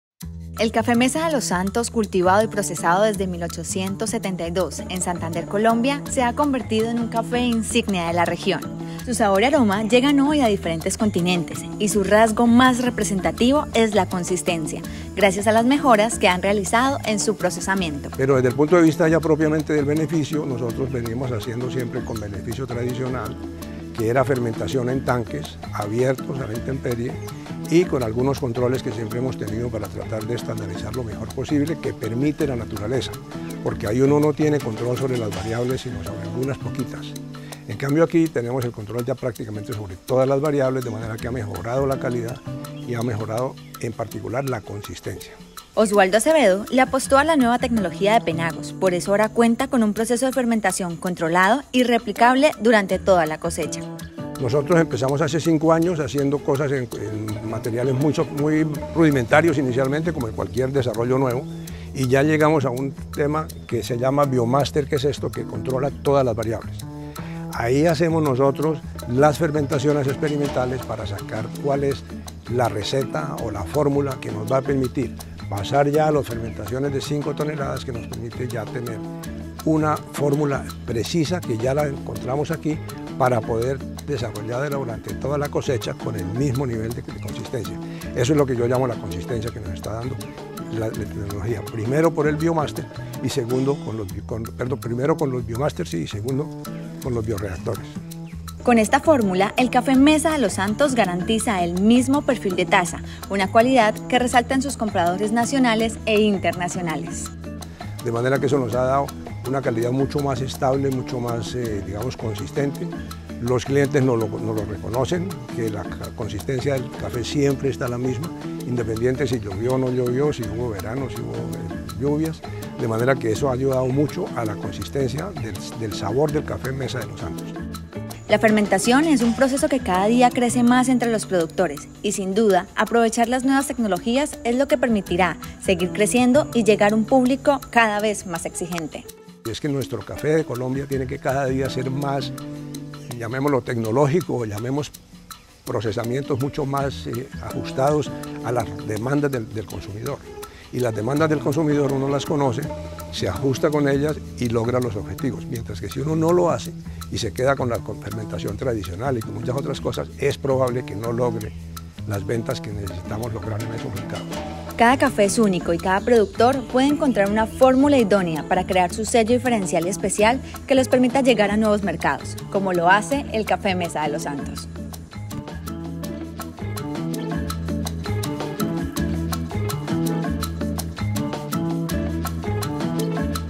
Café Mesa de los Santos: consistencia y calidad
¡Les compartimos el secreto detrás del Café Mesa de los Santos!☕
Con nuestros  equipos Penagos, esta empresa insignia de la región ha elevado su calidad. La consistencia del sabor y aroma son su sello distintivo👌
La fermentación en nuestros BioMáster y Biorreactores, se enfoca en lograr un café único.  ¡Usted también puede encontrar su fórmula con nuestros equipos!🚨
Let us share with you the secret of Mesa de los Santos coffee!
With our Penagos equipment, this region's flagship has improved its quality. Consistency of taste and aroma are its hallmark👌.
Our BioMaster and Bioreactors focus on achieving a unique coffee. With our equipment, you can find your formula too!🚨